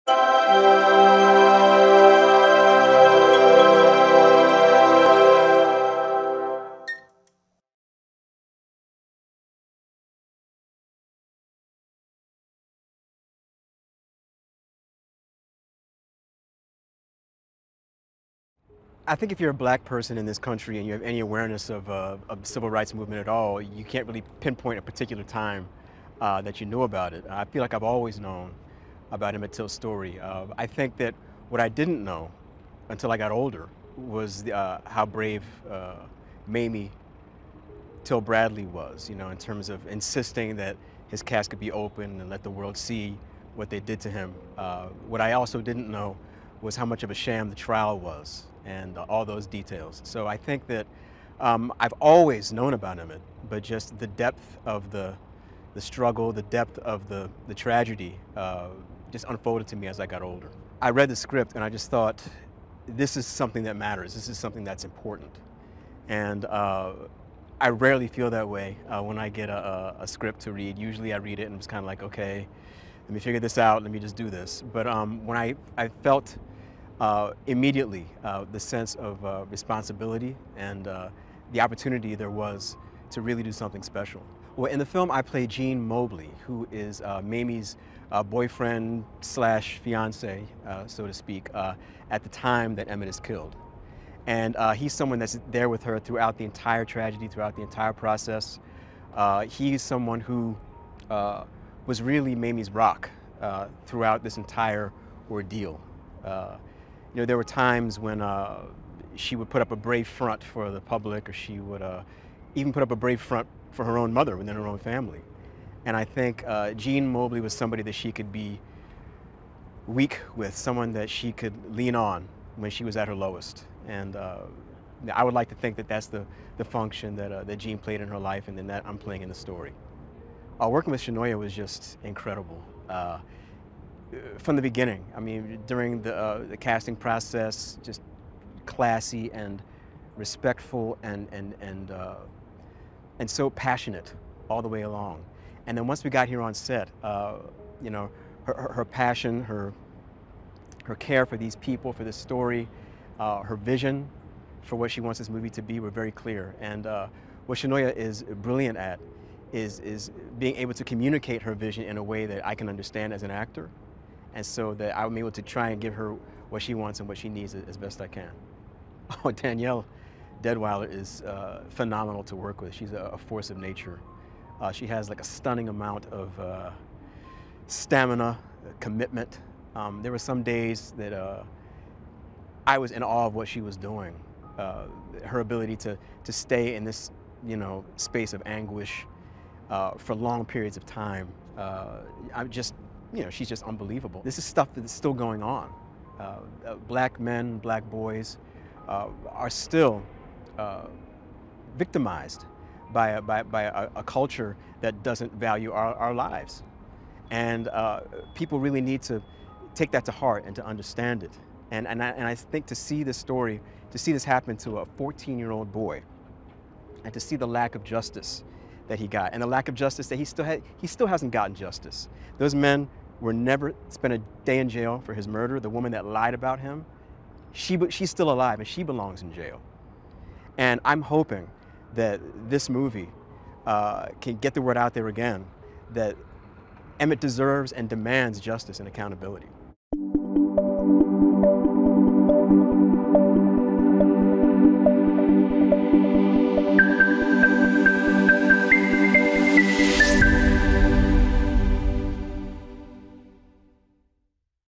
In-Depth Scoop | Sean Patrick Thomas - 'TILL'
'Till' actor Sean Patrick Thomas talks about what audiences can expect from the new film based on Emmett Till's true story.